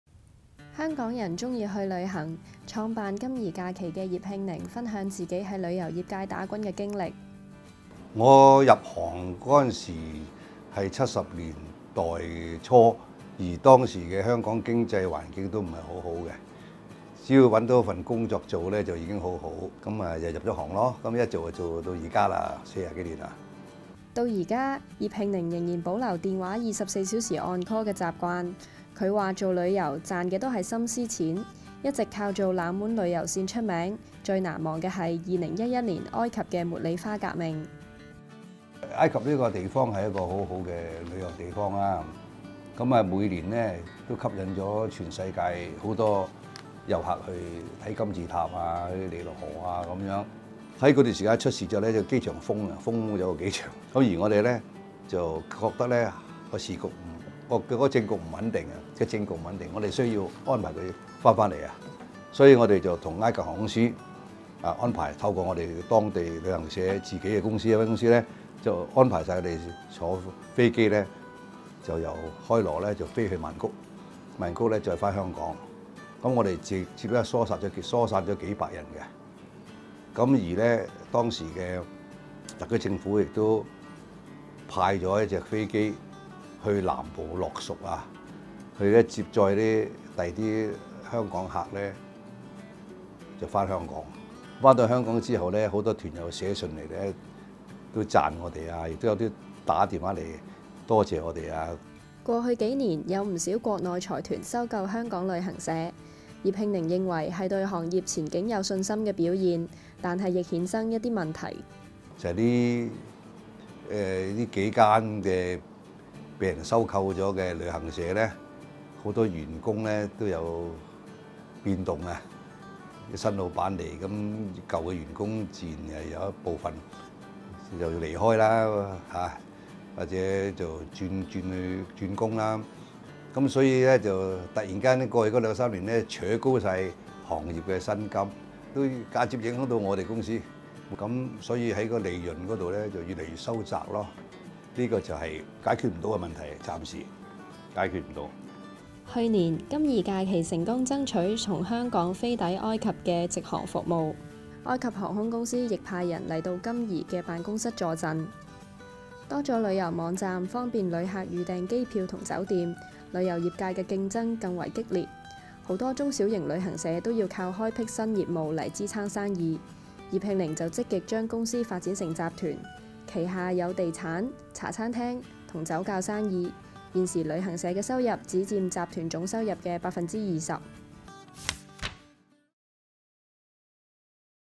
港旅行社拒財團收購 另闢新徑維持生意（2019年5月22日）
葉慶寧在旅遊界闖蕩近半世紀，創下多個第一，包括引入旅遊批發商，埃及、以色列、伊朗等冷門旅遊路線曾是「獨市」生意，贊助電視旅遊節目等，至今仍未捨得放手。他最難忘的是2011年埃及茉莉花革命一役，不用等特區政府包機，亦能將旅客安全送回香港。……（更多內容：